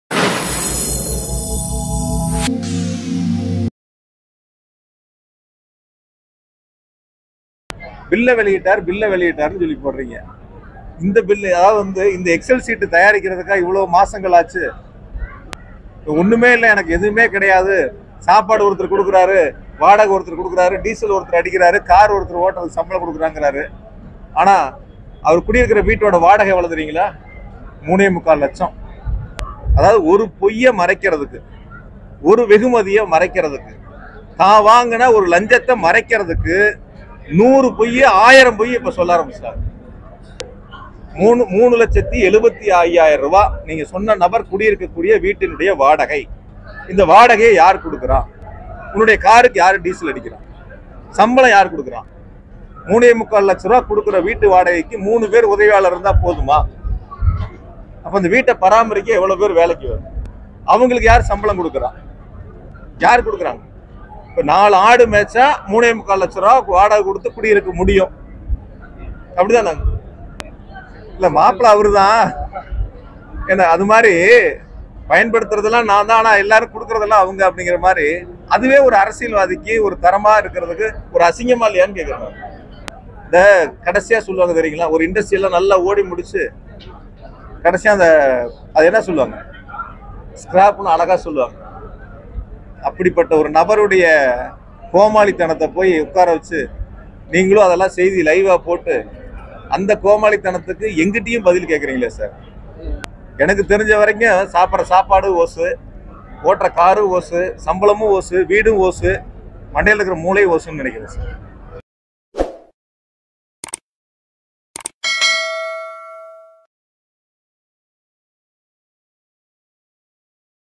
வீடுஓசி|கார்ஓசி|சோறு ஓசி|விளாசிய செந்தில் பாலாஜி
மாப்பிள்ளை அவர்தான்: சட்டை என்னது: படையப்பா பட காமெடி போல் அண்ணாமலை பிரஸ்மீட் இருந்ததாக செந்தில்பாலாஜி விளாசல்.